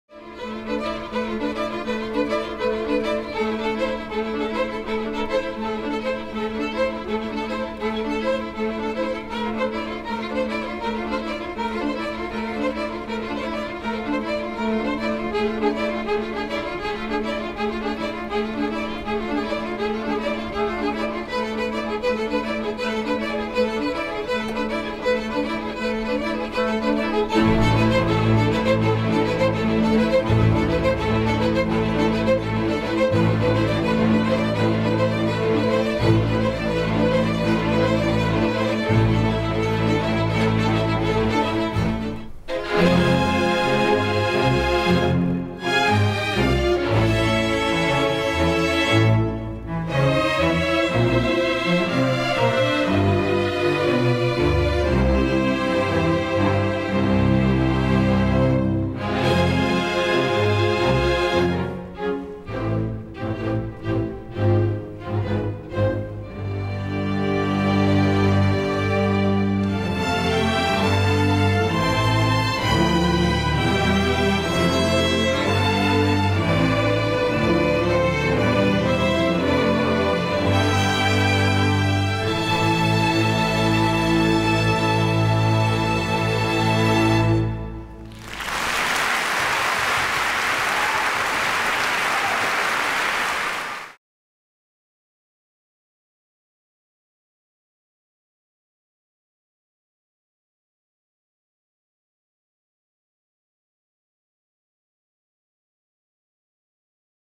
SF Ballet in Tomasson's "Fifth Season"
The 2014 Season Program 7 includes the return of Helgi Tomasson's masterful The Fifth Season.